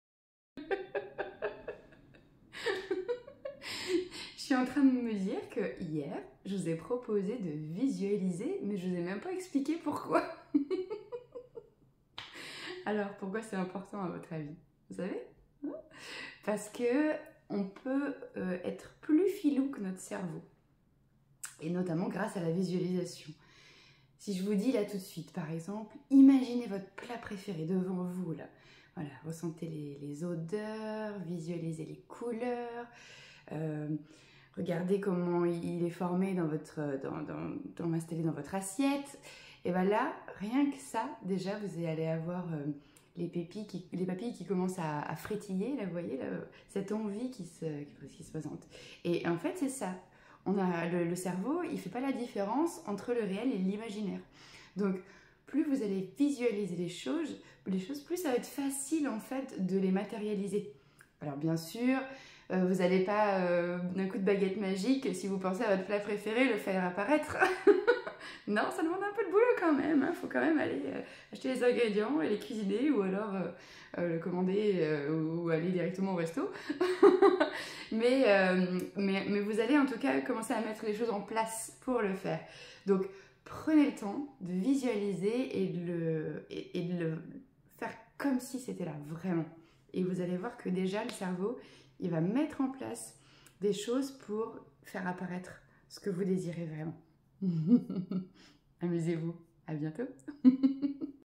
03/01/2023 ☀️ Pourquoi visualiser ? 💖
Je me rends compte que je n'ai pas expliqué pourquoi il était intéressant de visualiser 😅

Alors je vous l'explique en ce 3 janvier 2023 ✨ Allez c'est cadeau : Joyeux Noël 😂